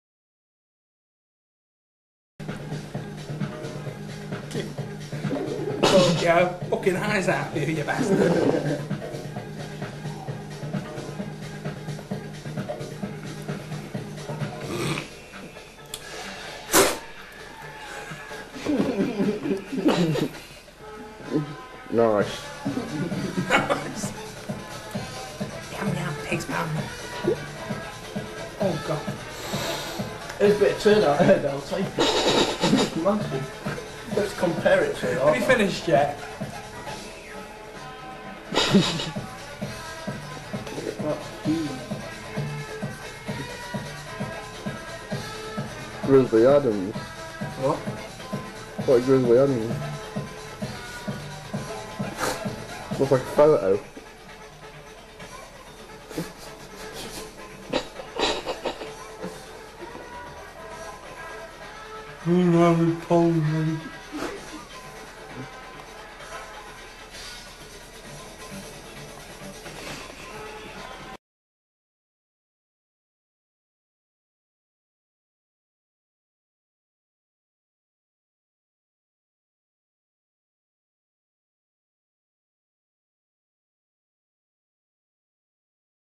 Olly of Old
A very Old clip of video - me about 15 years ago - just messin' about with some mates ( as ya do!!)
Its a bit silly but fun